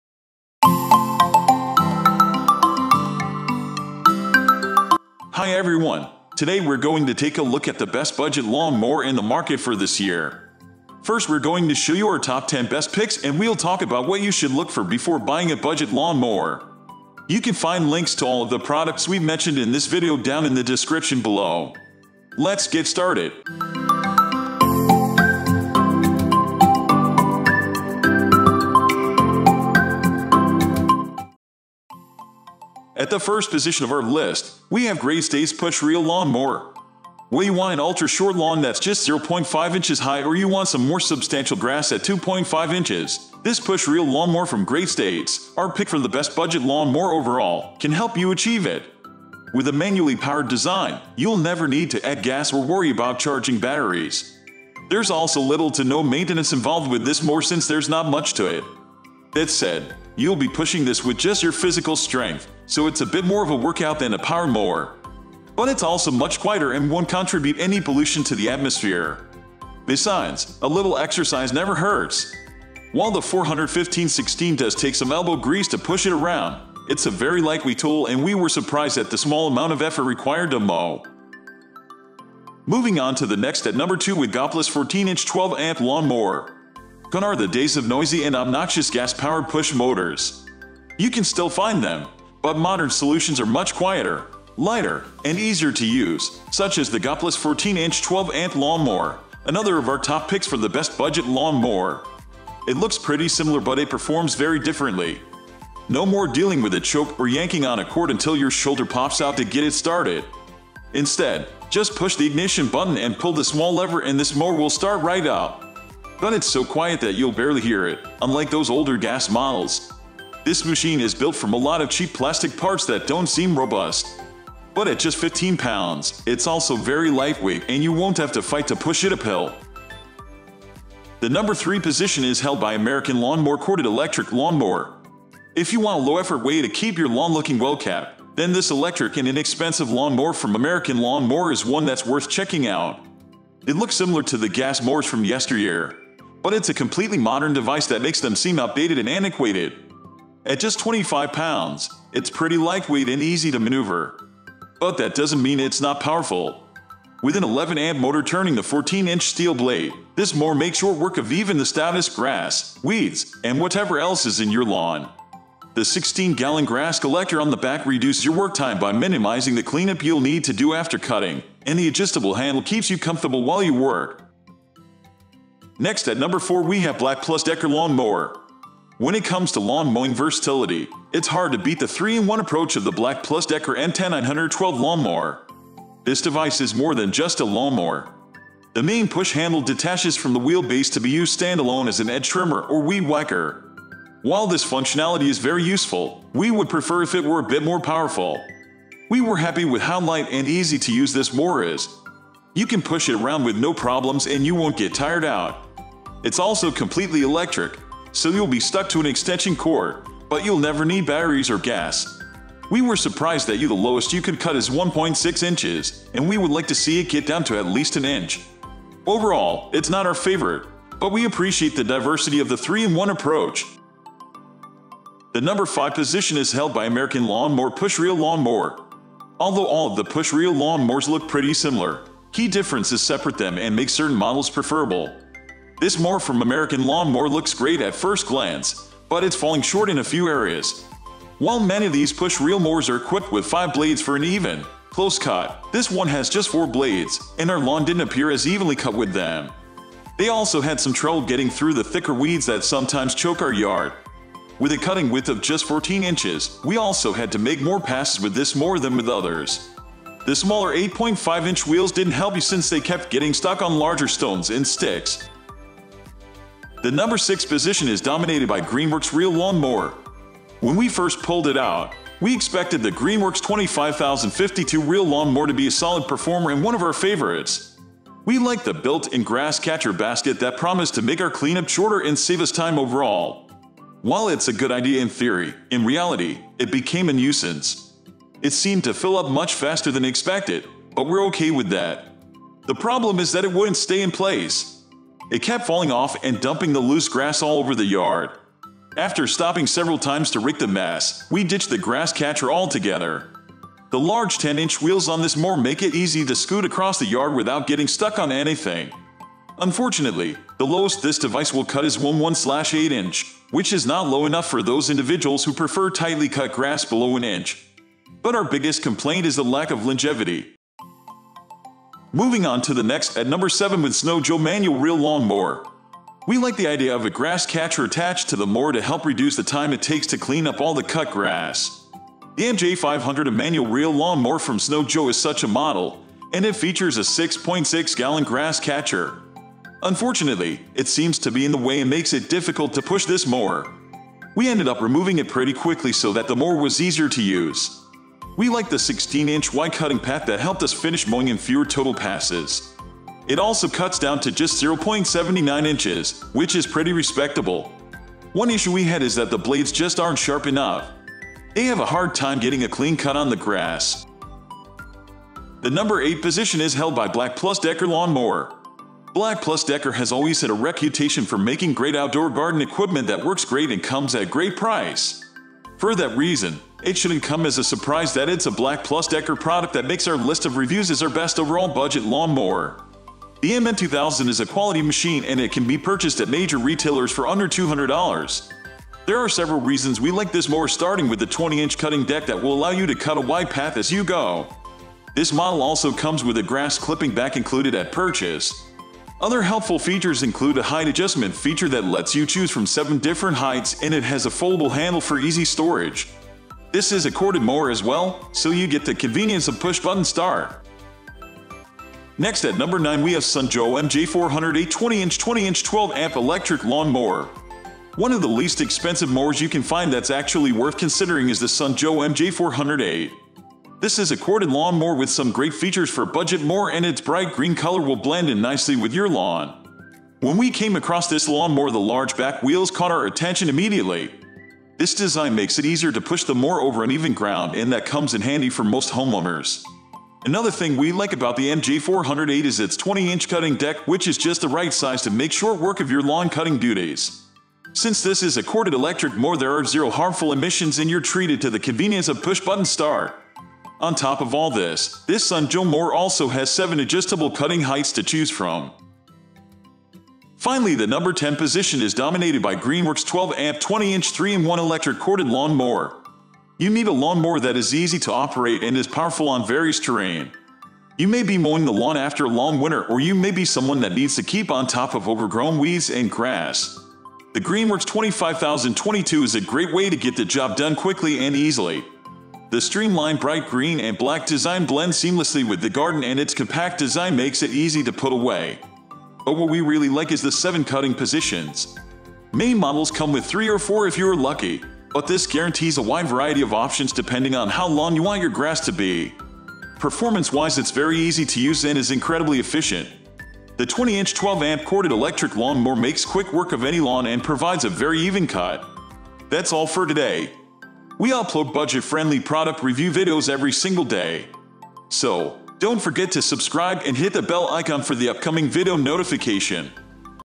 Top 10 Best Budget Lawn Mower (2023)
Links to the Best Lawn Mowers we listed in today's Lawn Mowers review video:

1 . Great States Push Reel Lawn Mower

2 . Goplus 14-Inch 12-Amp Lawn Mower

3 . American Lawn Mower Corded Electric Lawn Mower

4 . BLACK+DECKER Lawn Mower

5 . American Lawn Mower Push Reel Lawn Mower

6 . Greenworks Reel Lawn Mower

7 . Snow Joe Manual Reel Lawn Mower

8 . BLACK+DECKER Lawn Mower

9 . Sun Joe MJ408E 20 Inch 20-Inch 12-Amp Electric Lawn Mower

10 . Greenworks 12 Amp 20-Inch 3-in-1Electric Corded Lawn Mower

What are the Best Lawn Mowers in 2023?
In today's video we reviewed the Top 10 Best Lawn Mowers on The Market in 2023. We made this list based on our personal opinion and we ranked them in no particular order, after doing our research based on their prices, quality, durability, brand reputation and many more.

After watching this video you will know which are the best budget, top selling and top rated Lawn Mowers on the market today.

If you choose from this list you can be sure that you buy one of the best Lawn Mowers available today.